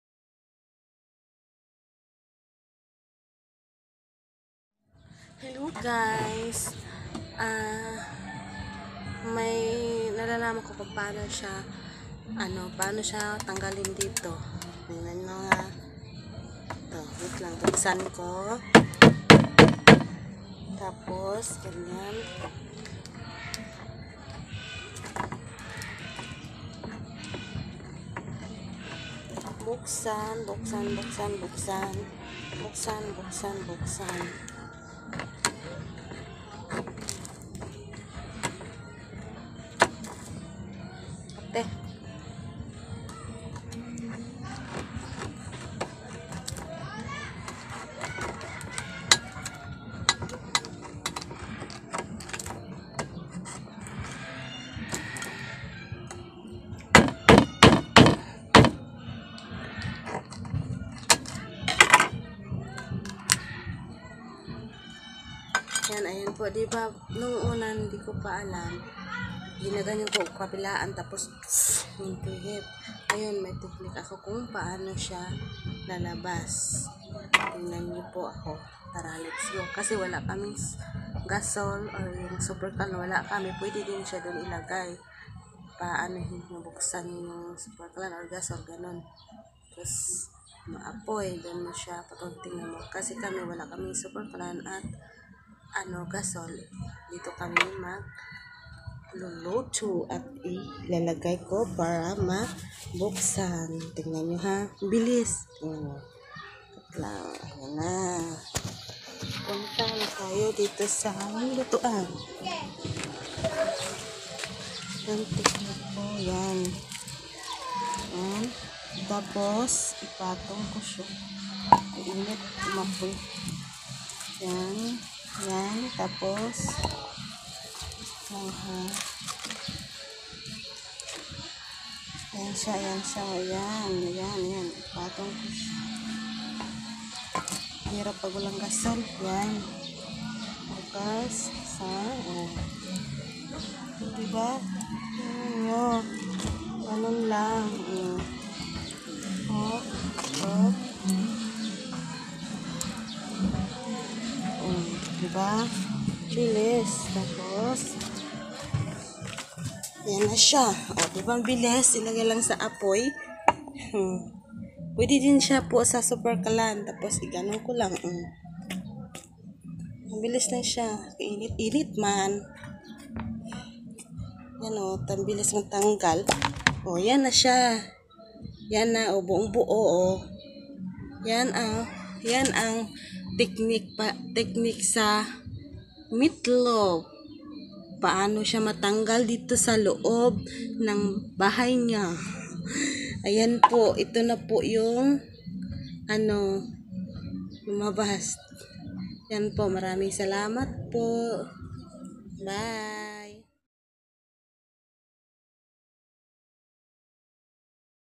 Paano palabasin ang meatloaf sa lata in easy way, share and tutorial muna sa aming bahaykubo ytc,,,
share lang aking asawa ang mabilis na paraan kung papaano palabasin ang meatloaf sa lata ng mabilis at hindi hinihipan...

salamat sa DIYOS